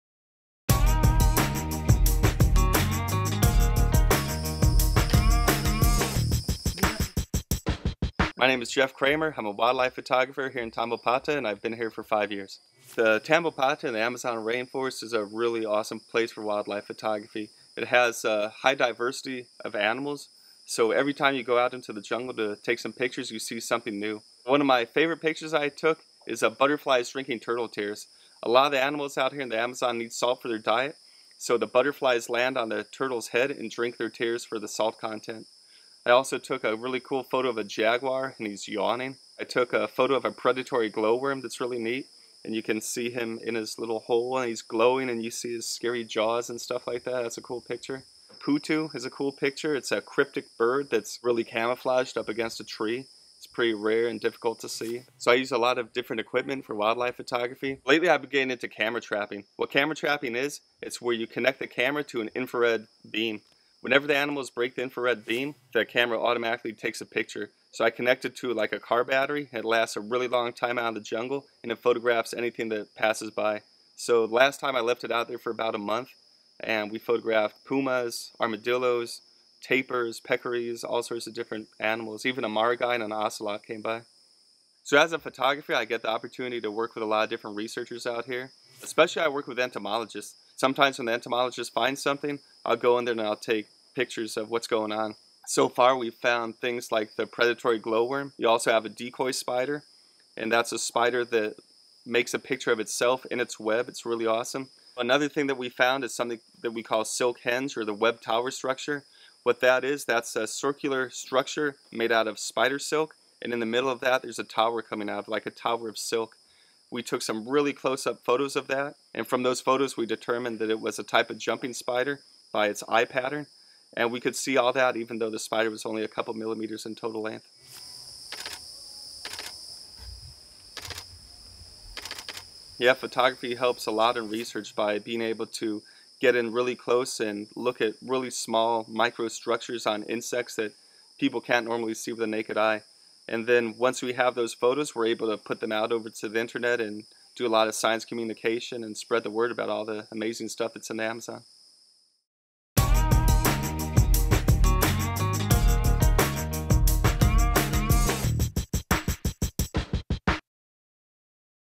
Wildlife Photography in Tambopata in Peru
This video is about wildlife photographer Jeff Cremer's work in the Tambopata Research Center in the Peruvian Amazon.